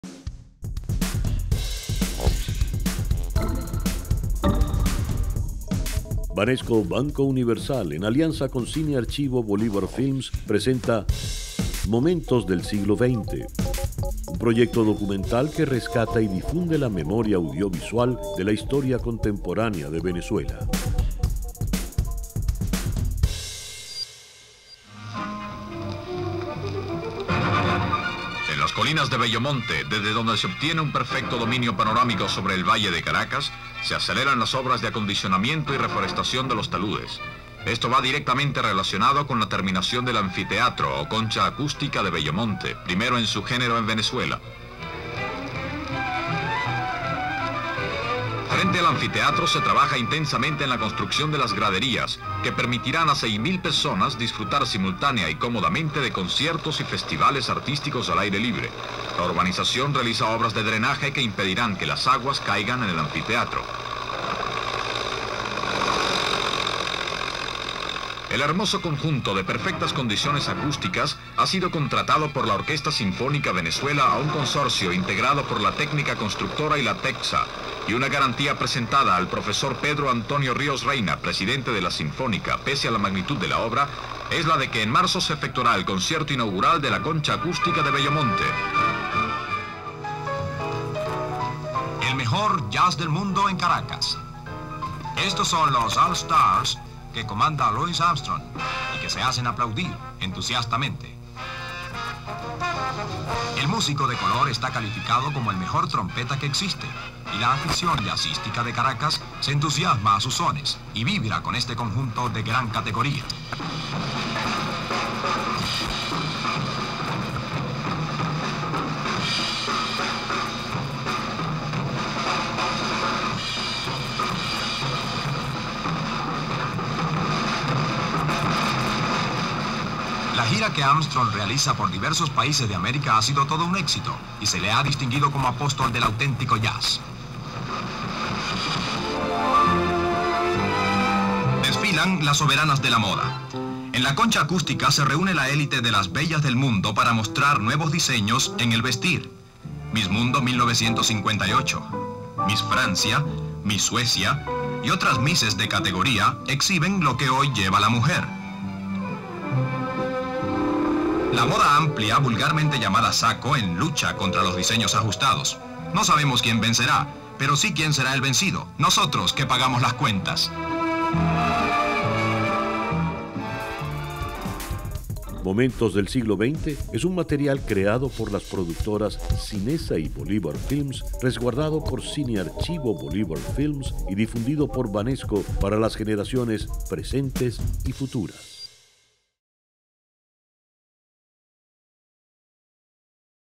Momentos del siglo XX: La Concha Acústica de Bello Monte
Banesco Banco Universal, en alianza con Cine Archivo Bolívar Films, presenta Momentos del siglo XX, un proyecto documental que rescata y difunde la memoria audiovisual de la historia contemporánea de Venezuela.

En esta entrega, se muestran las imágenes de la construcción de la Concha Acústica José Ángel Lamas o Concha Acústica de Bello Monte, además de otros momentos que se convirtieron en noticia. Hoy este material es parte de la memoria histórica y cultural de Caracas en el siglo XX.